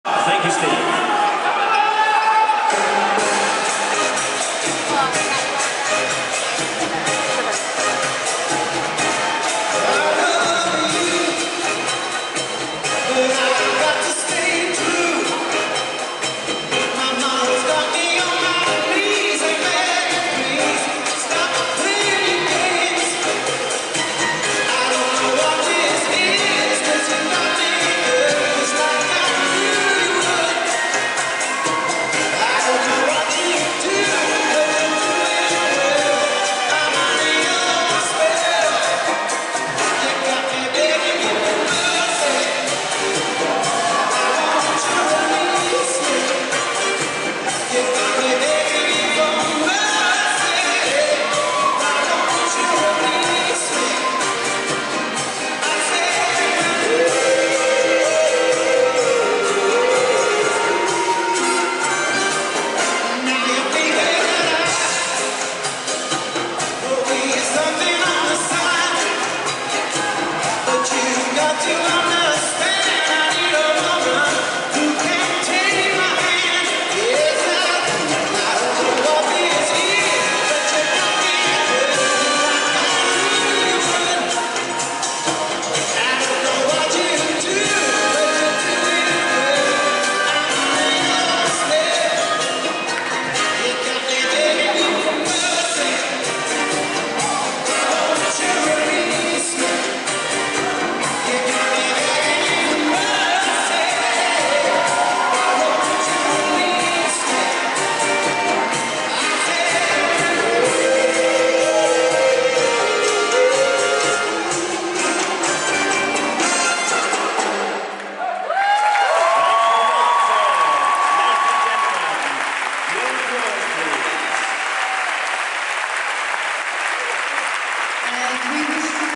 International Championships 2012, Grand Final, Professionals Latin, Cha-Cha-Cha
Riccardo Cocchi & Yulia Zagoruychenko, Michal Malitowski & Joanna Leunis, Sergey Sourkov & Agnieszka Melnicka, Maurizio Vescovo & Andra Vaidilaite, Andrej Skufca & Melinda Torokgyorgy, Stefano Di Filippo & Dariya Chesnokova